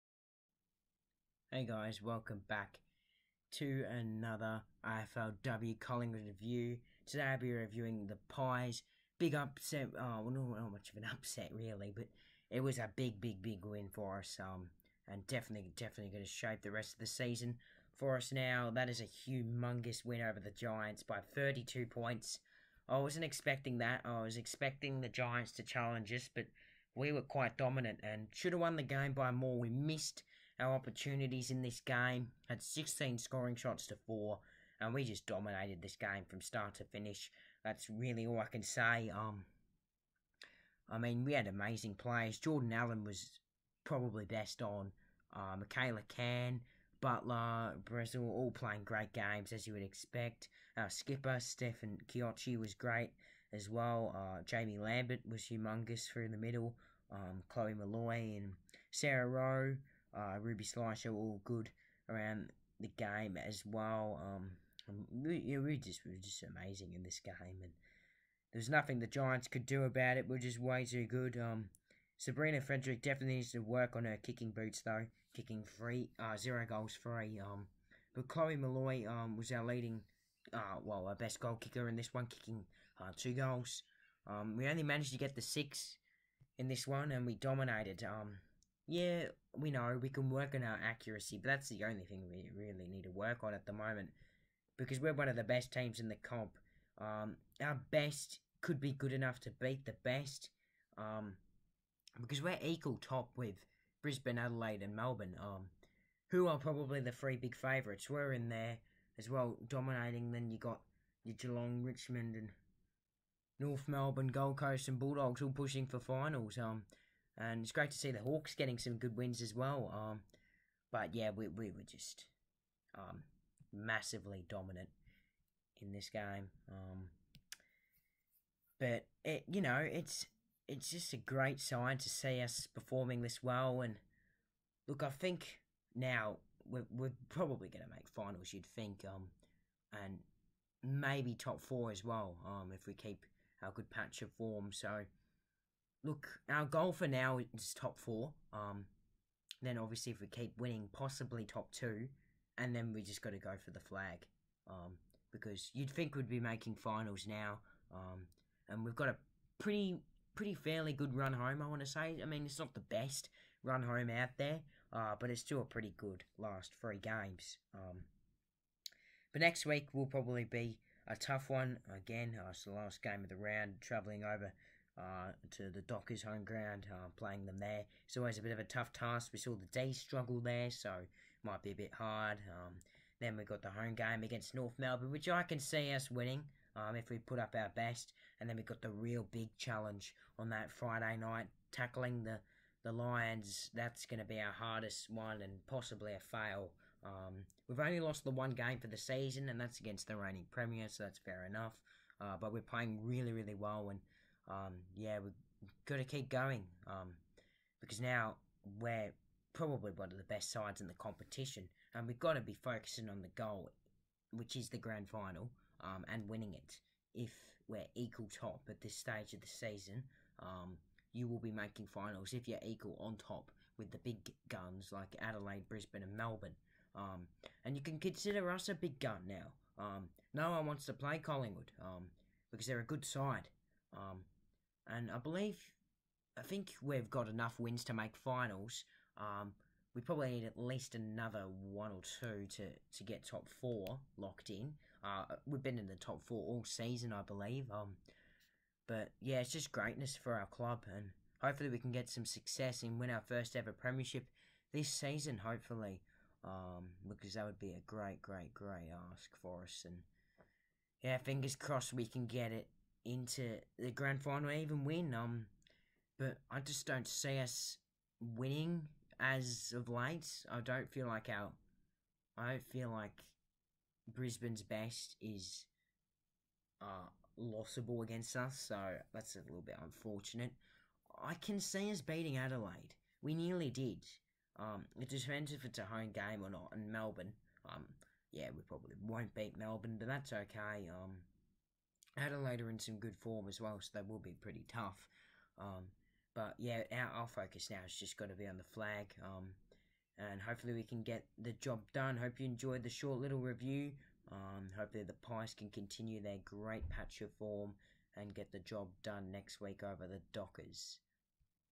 Are the pies flag contenders? Collingwood AFLW review!
Another great win that puts us equal on top with other big sides!